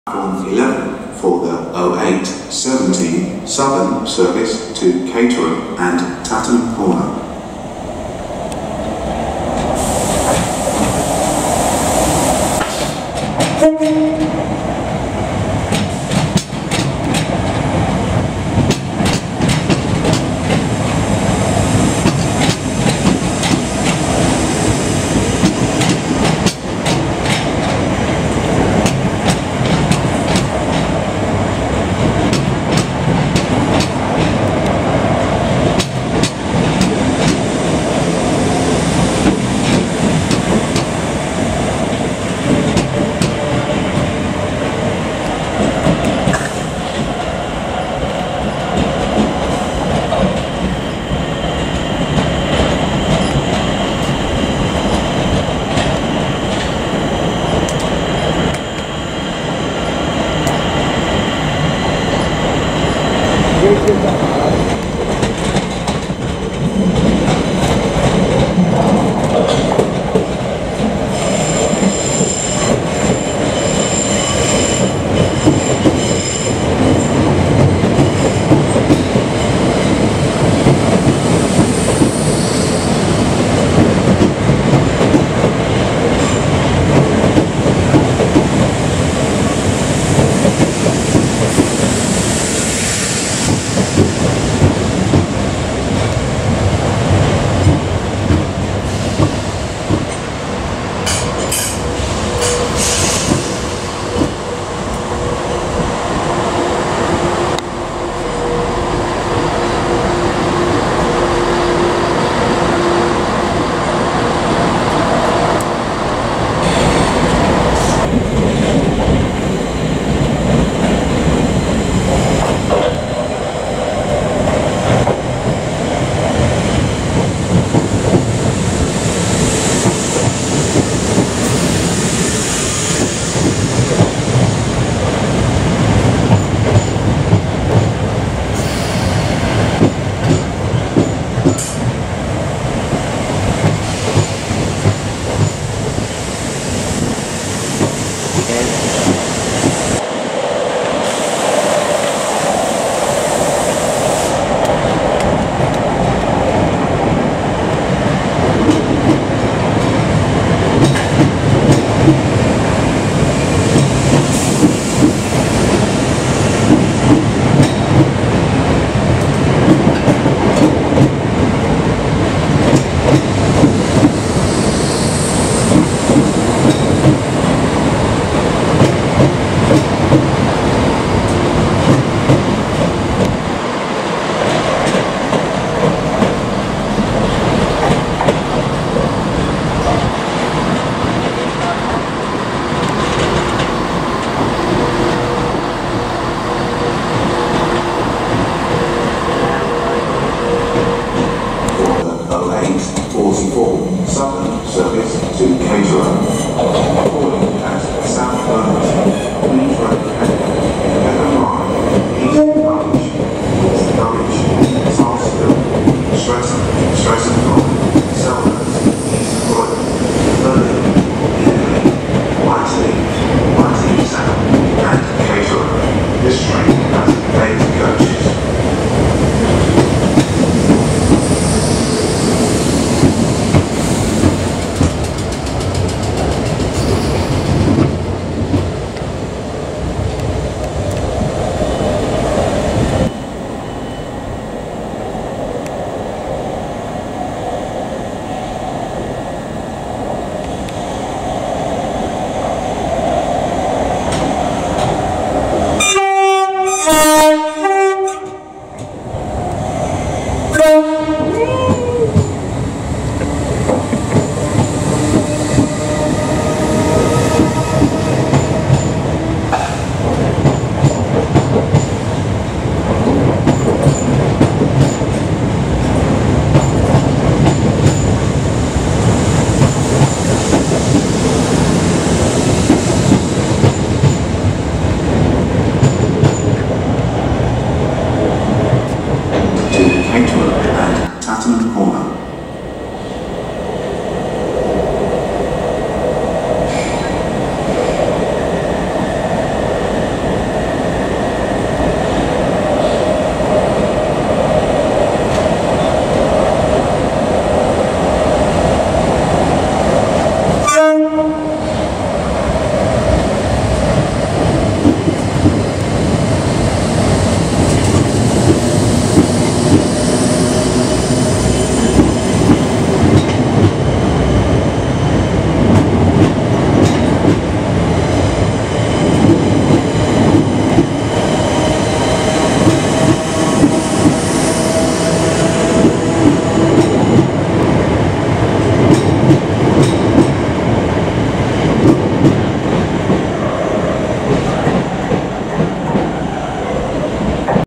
Southern Class 455s @ London Bridge During Rush Hour - 12th May 2022
In This Video: Southern Class 455s @ London Bridge During Rush Hour.